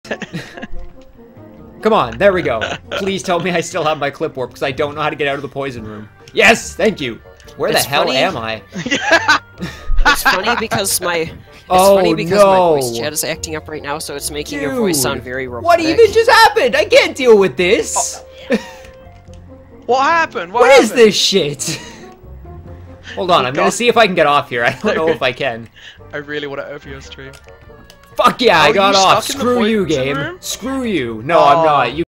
A Warm Welcome Poison Clip Fail
After getting out of the Poison room we clipped to an interesting location...